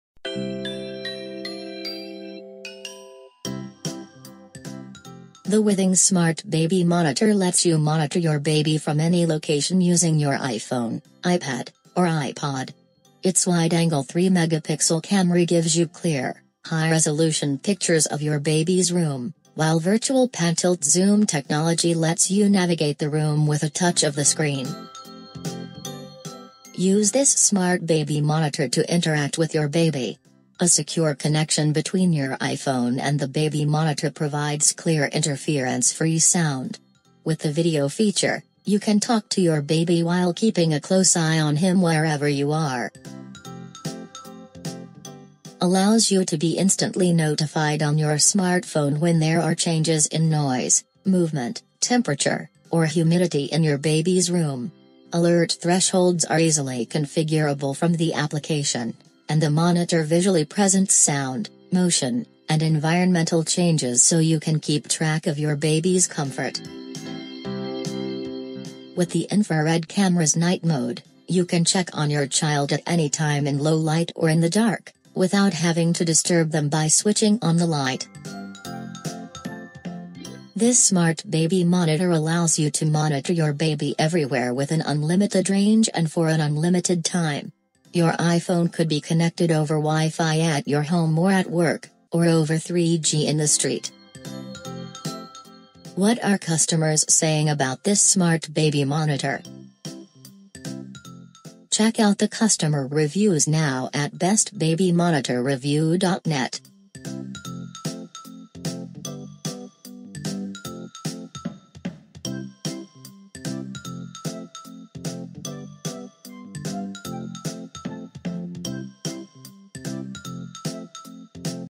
Withings Smart Baby Monitor Review
The Withings Smart Baby Monitor lets you monitor your baby from any location using your iPhone, iPad, or iPod. Its wide-angle 3-megapixel camera gives you clear, high-resolution pictures of your baby's room, while virtual pan-tilt-zoom technology lets you navigate the room with a touch of the screen.

- Talk, Play, and Interact with Your Baby
Use the Smart Baby Monitor to interact with your baby. A secure connection between your iPhone and the baby monitor provides clear, interference-free sound. With the video feature, you can talk to your baby while keeping a close eye on him wherever you are.

- Alerts for Noise, Motion, and Temperature Changes.
The WithBaby app allows you to be instantly notified on your smartphone when there are changes in noise, movement, temperature, or humidity in your baby's room. Alert thresholds are easily configurable from the app, and the monitor visually presents sound, motion, and environmental changes so you can keep track of your baby's comfort.

- Monitor in the Dark with Night Mode
With the infrared camera's night mode, you can check on your child at any time, in low light or in the dark without having to disturb them by switching on the light.

- Monitor at Home or While Away
The Smart Baby Monitor allows you to monitor your baby everywhere with an unlimited range and for an unlimited time. For example, your iPhone could be connected over Wi-Fi at your home, over 3G in the street or over Wi-Fi at your place of work. It can also be connected via an Ethernet cable or via Bluetooth in locations without an Internet connection